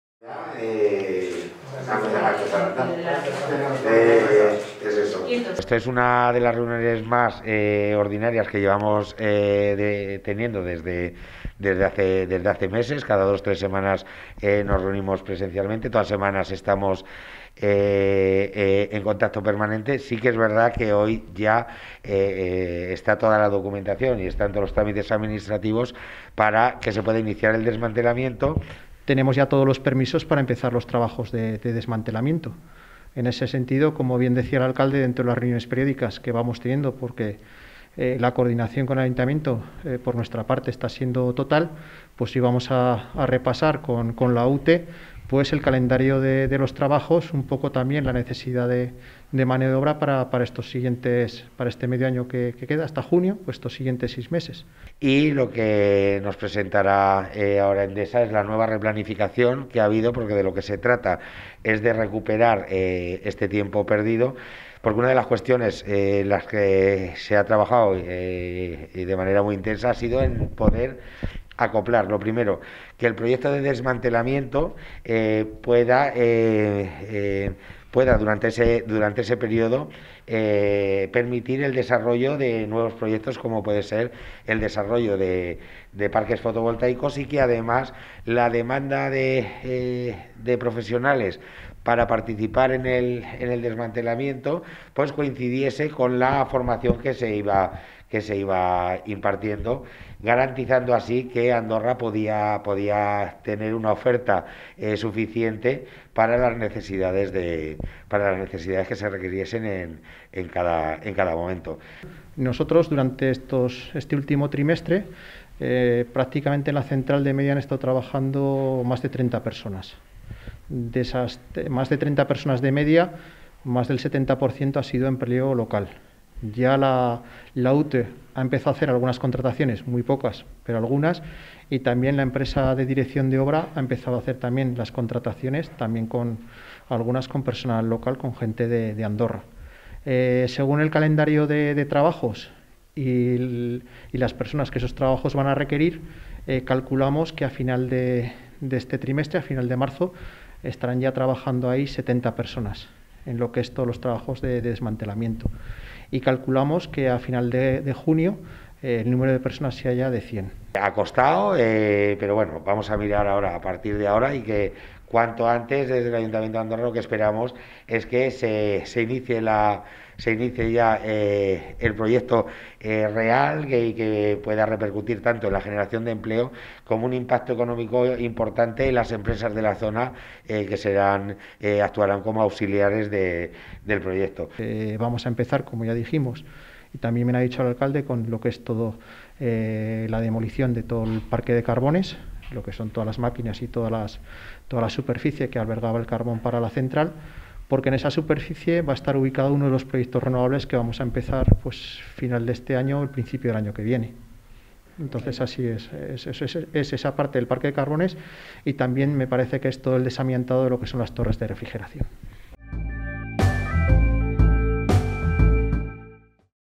Reunión para el desmantelamiento de la Térmica de Andorra
En la tarde del jueves 4 de febrero se han reunido en el ayuntamiento de Andorra para tratar el desmantelamiento de la Central Térmica de Andorra. Entre otras personas ha asistido el director general de Endesa Aragón, Ignacio Montaner y el alcalde de la localidad, Antonio Amador.